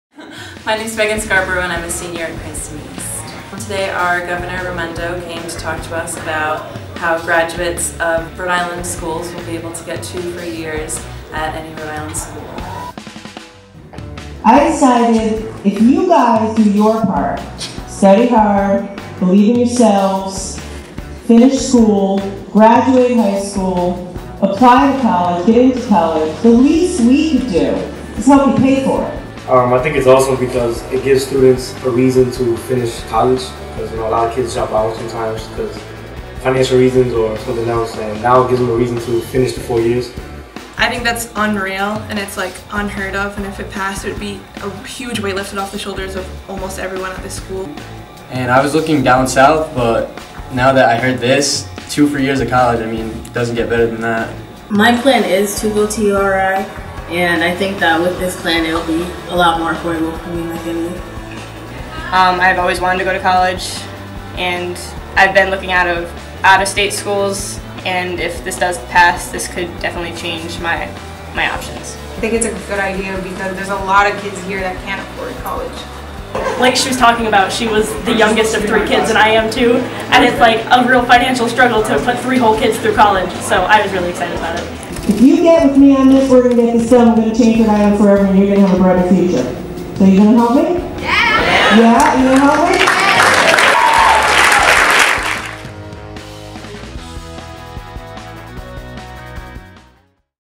A New Promise for a New World
Governor Gina M. Raimondo visited Cranston East High School to tell students about her plan to provide two free years of college at URI, CCRI or RIC.